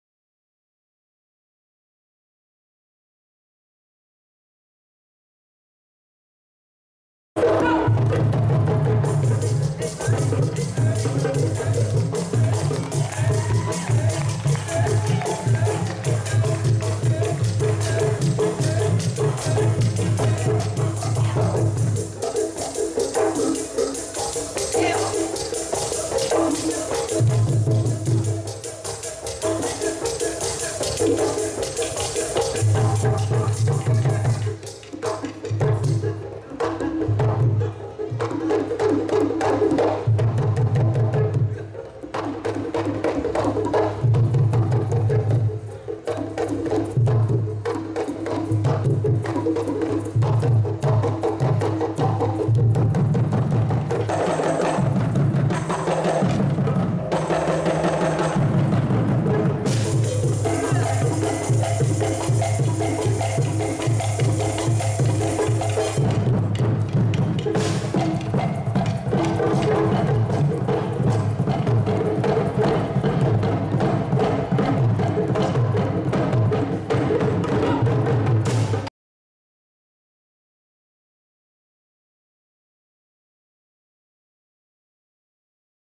HUT AF 15 part 1
Pada peringatan Hari Ulang Tahun Yayasan  ANGGI (ANGGI Foundation/AF) ke-15, AMAN Perkusi dan Areli sempat berkolaborasi dengan seniman muda Rizal Abdul Hadi, menampilkan karya bernuansa tradisi sekaligus menghibur anak-anak dan penggiat kegiatan sosial.